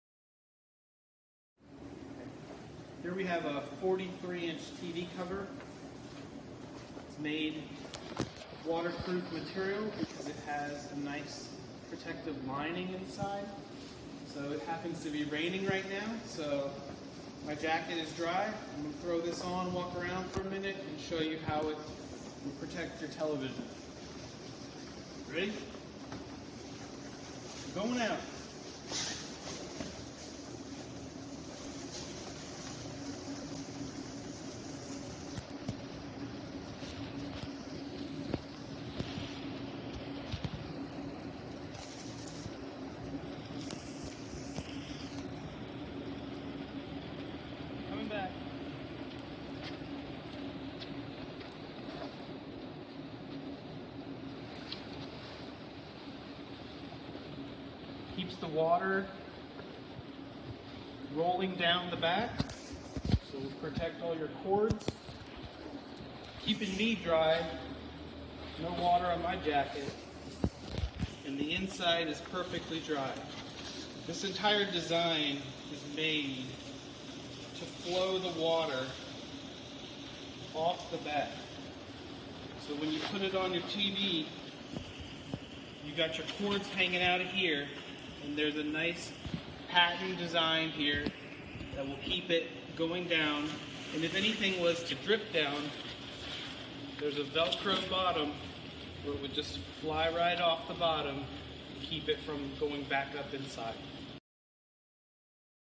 Stronghold Tv covers Waterproof test
Looking for a Premium quality waterproof outdoor TV cover? Look no further! Our latest test results show that our covers can protect your TV from water damage, even in extreme conditions.
Don't take any chances with your expensive TV - get a cover today and rest easy knowing your investment is safe!